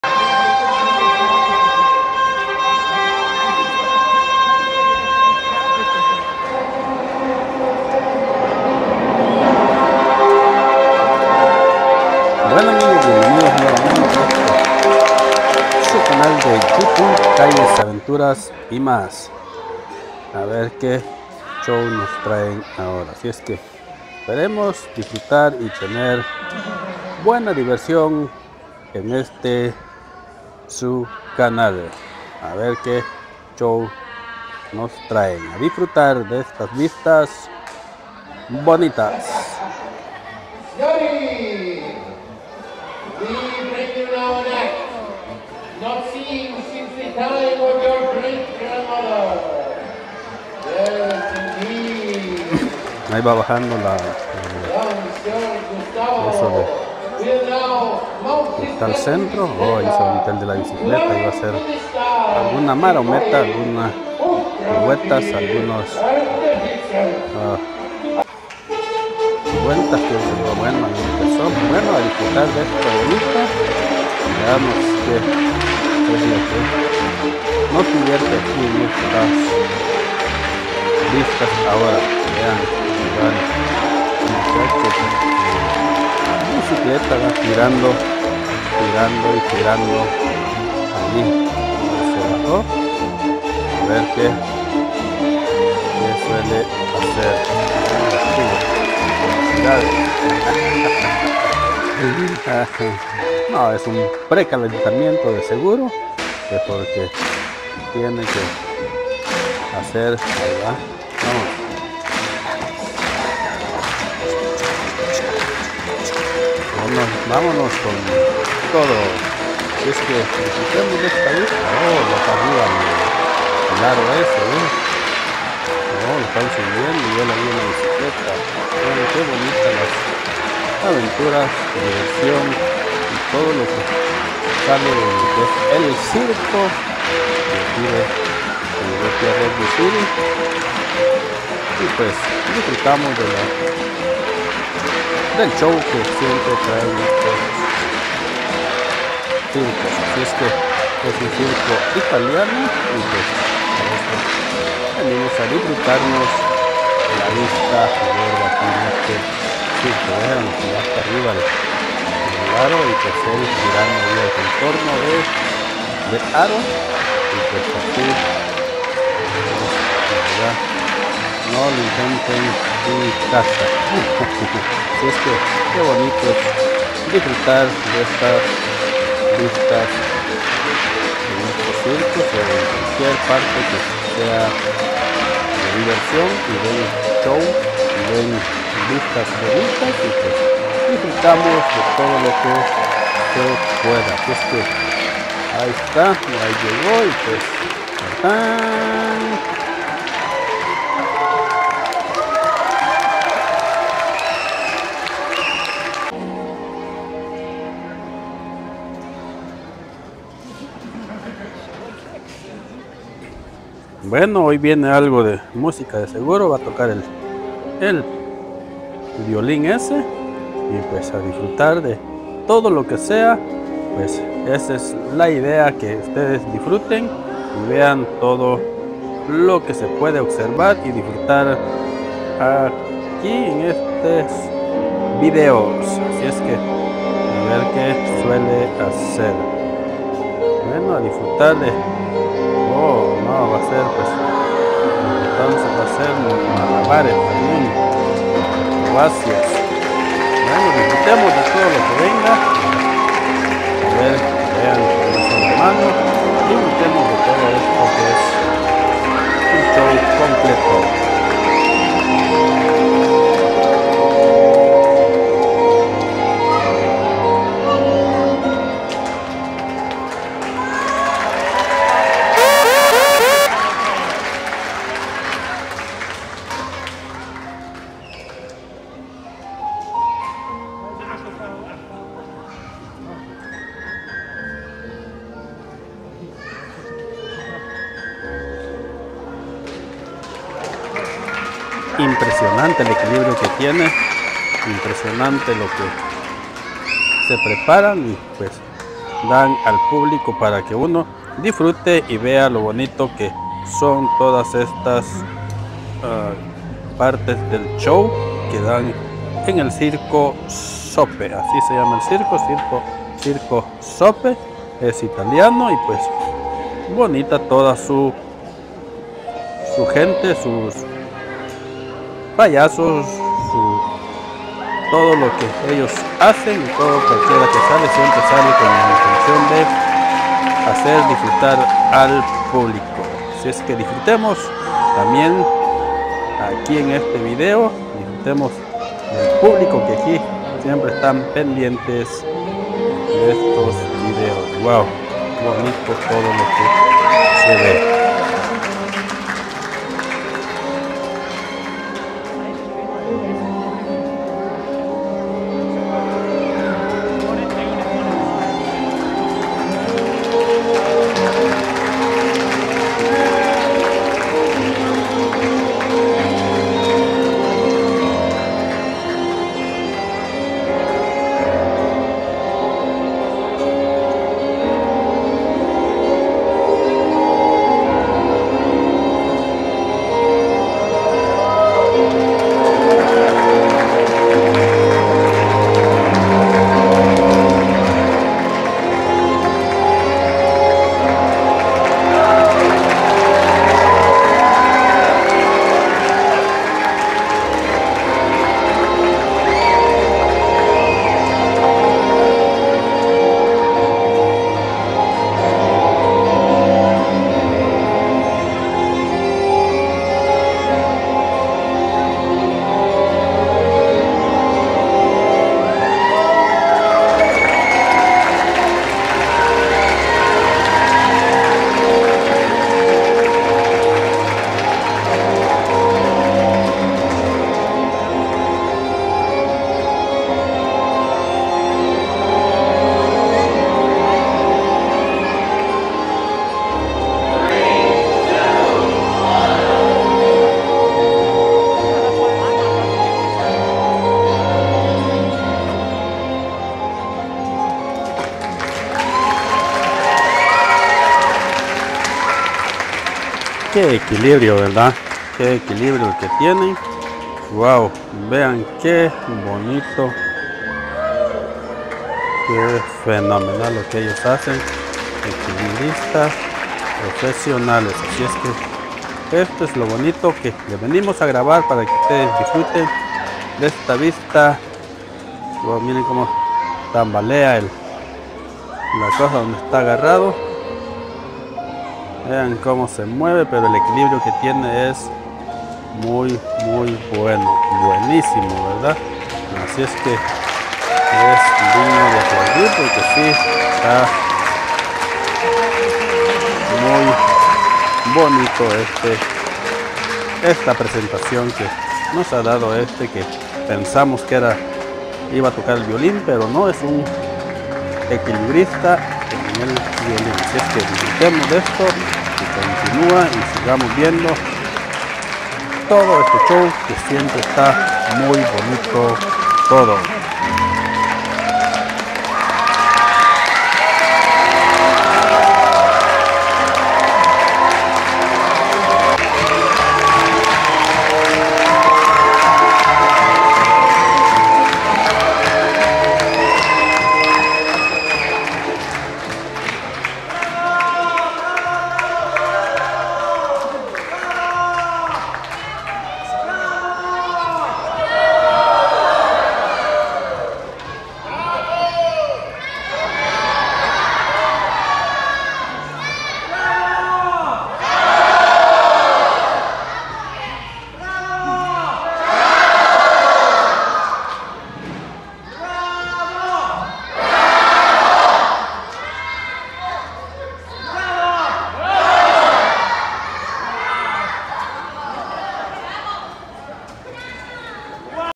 Circo 🤡 Sope muy bonito y divertido
Hoy estuvo más divertido y emocionante el show que nos vimos en el circo 🤡